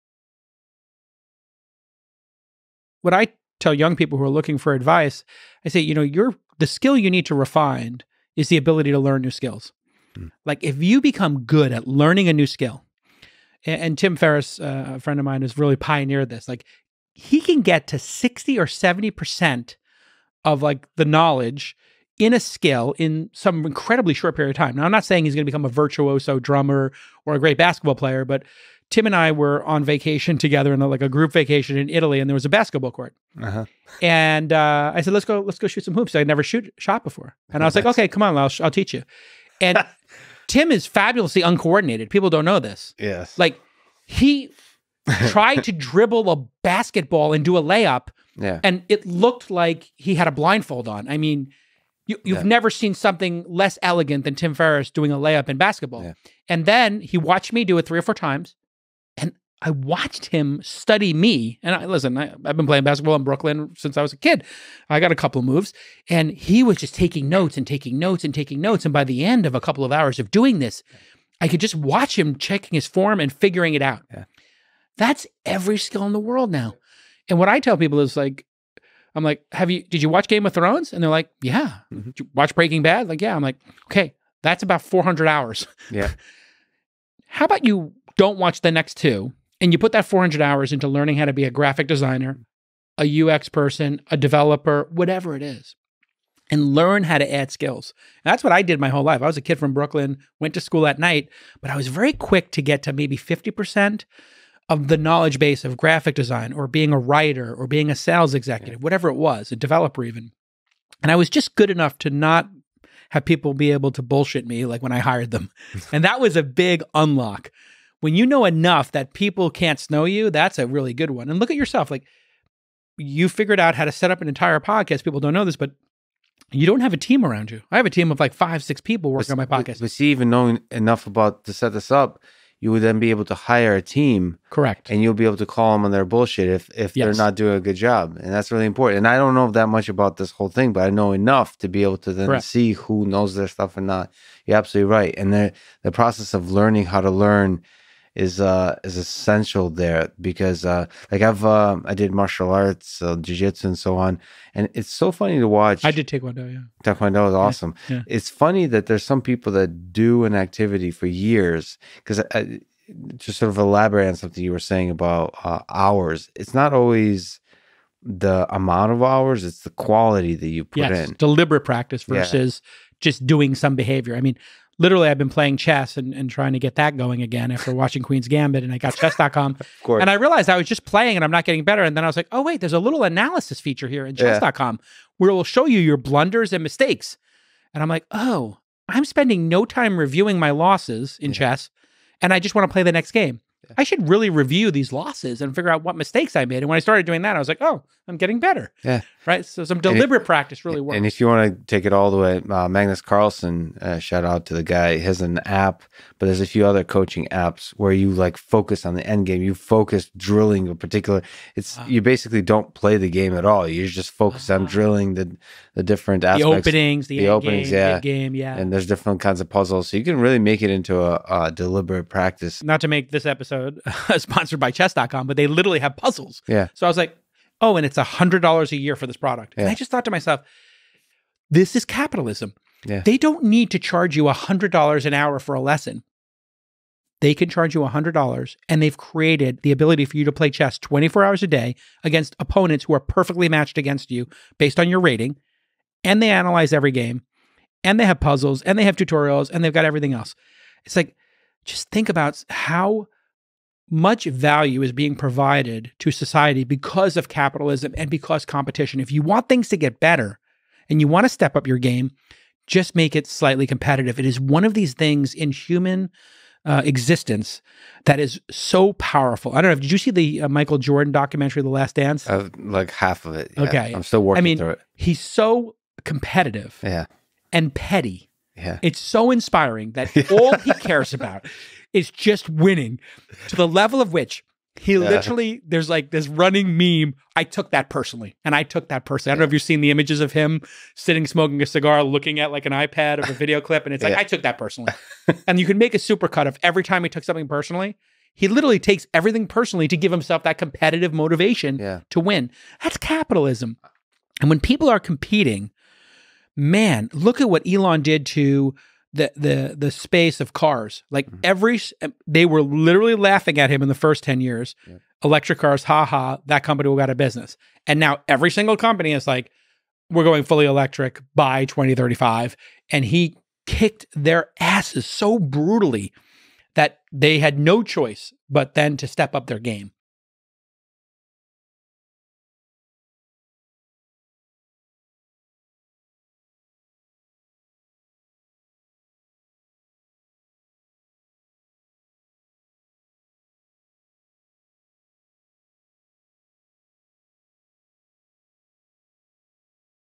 Deliberate Practice: Learning to learn is the most important skill | Jason Calacanis and Lex Fridman
GUEST BIO:
Jason Calacanis is an angel investor, entrepreneur, and co-host of All-In Podcast and This Week in Startups.